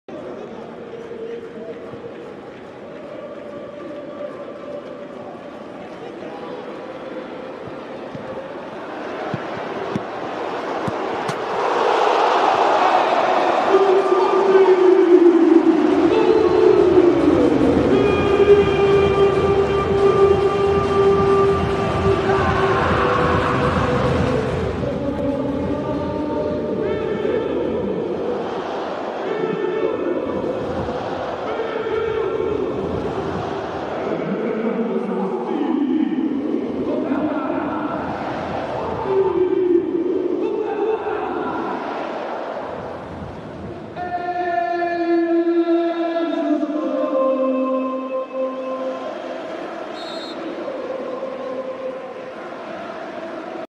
Golo Pedro Gonçalves: Sporting (1)-0 Tondela (Taça de Portugal 23/24)
Léo Navacchio afasta o cruzamento de Gyoeres, Nuno Santos aparece para a recarga, mas remata prensado contra Lucas Mezenga, a bola sobra para Pedro Gonçalves que atira à vontade para o 1-0!